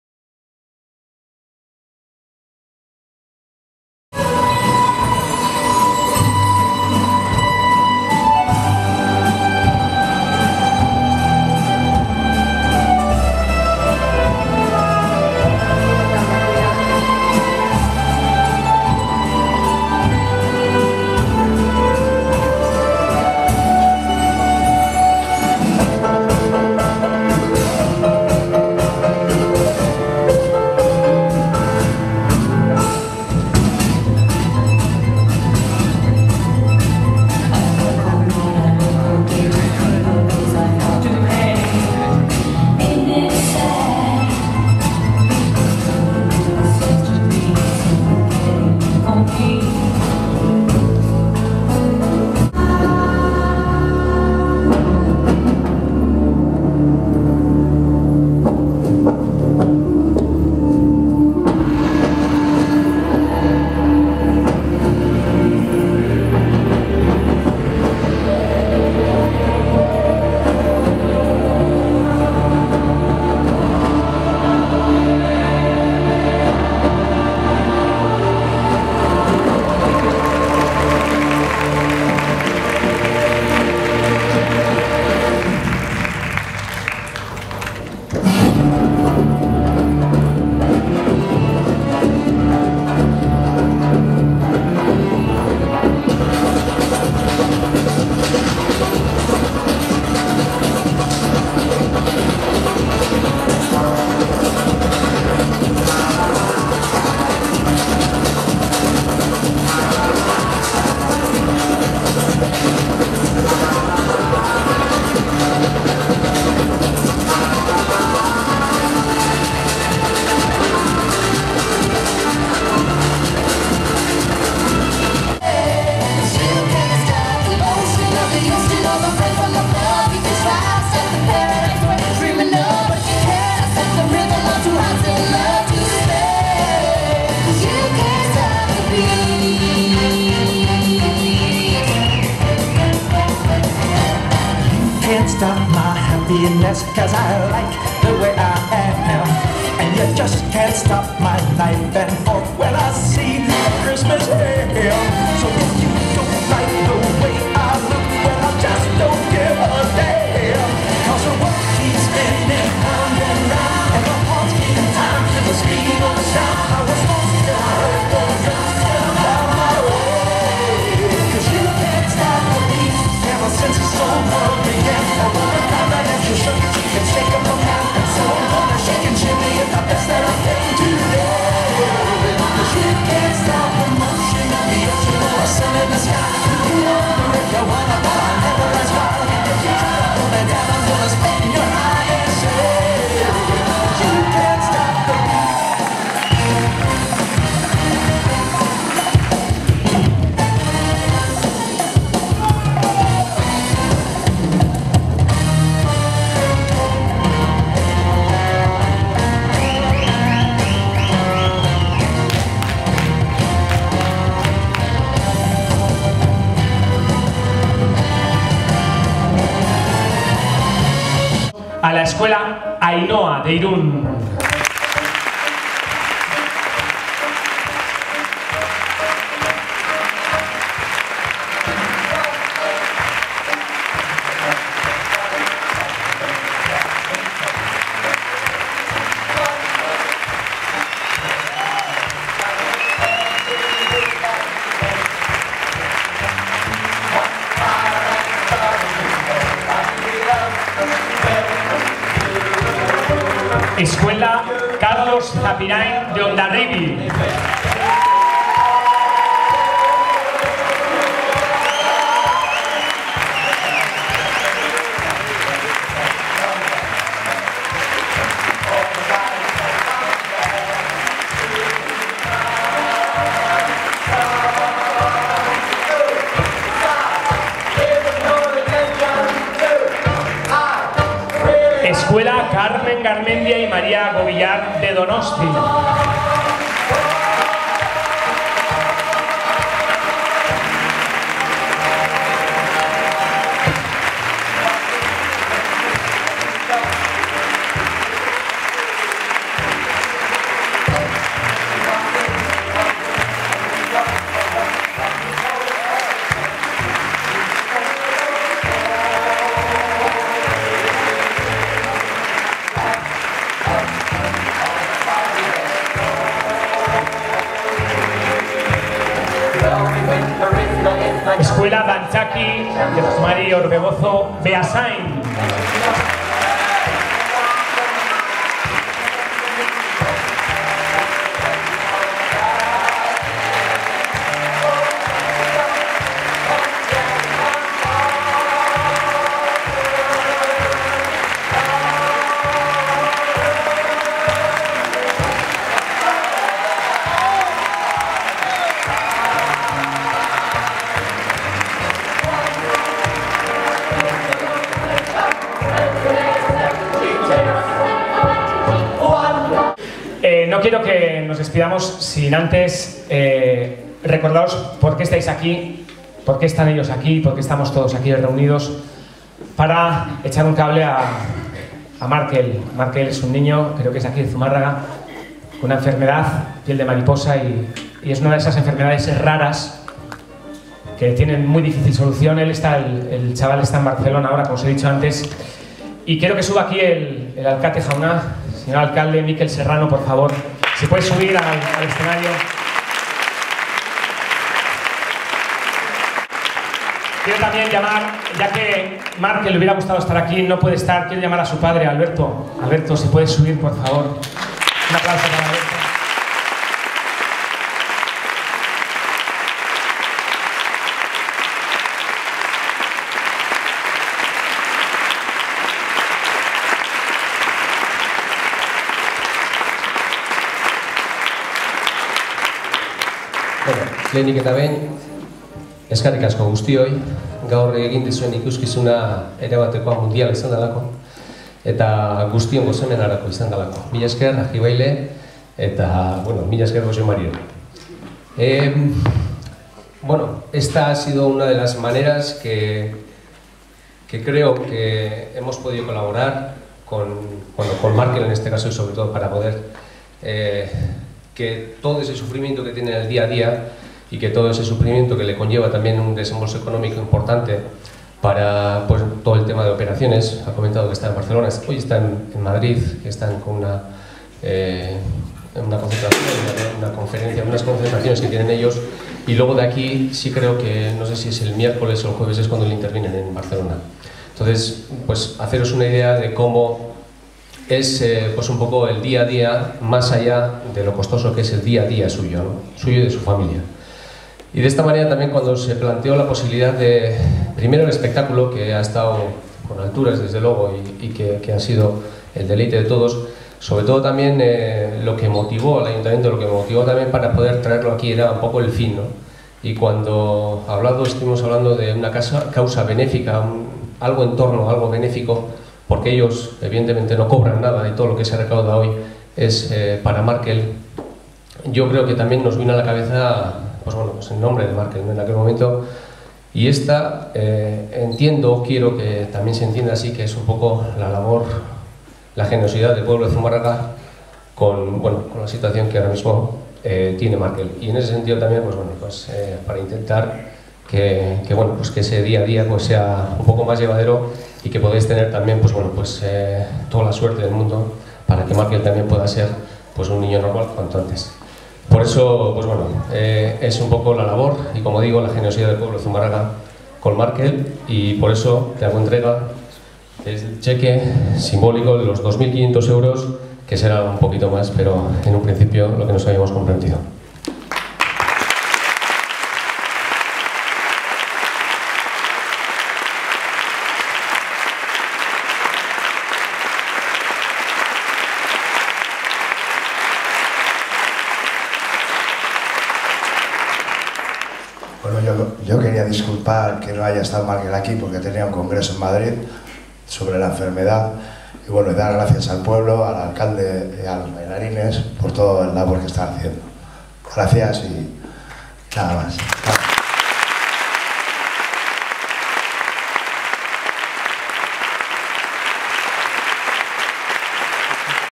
Markelen aldeko saloi-dantza erakustaldia. 2011 urria (2)
Markel Lopez mutikoaren aldeko saloi-dantza jaialdia izan da gaur Zelai Arizti zineman. 2011 urria (Bigarren zatia)
Espectáculo de bailes de salón en el cine Zelai Arizti, en favor del niño Markel López. Octubre de 2011 (Segunda parte)